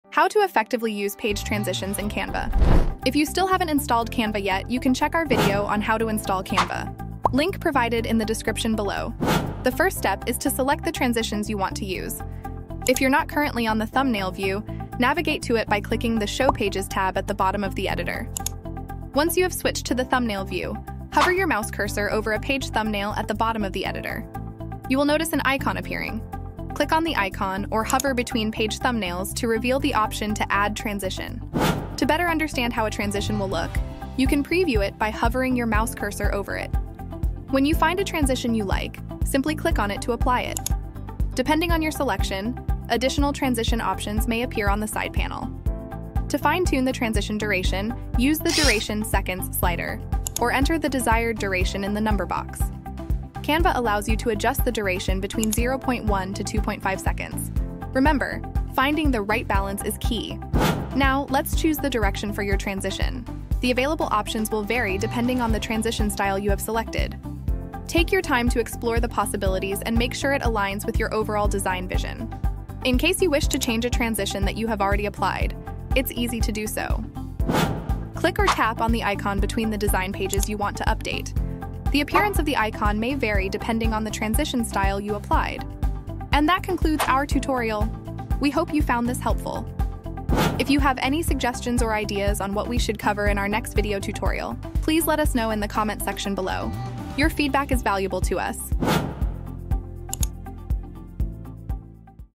Amazing Page Transitions In Canva
Time Stamp:
0:00 Intro
0:02 How to install canva
0:09 Add transition
0:46 Add duration
1:01 Add direction
1:28 Outro

Video Description:
In this tutorial, learn how to use page transitions in Canva to add a touch of professionalism and engagement to your designs. Follow along as we show you the step-by-step process, from selecting transitions to adjusting their duration and direction. Enhance your visual creations with smooth and captivating page transitions!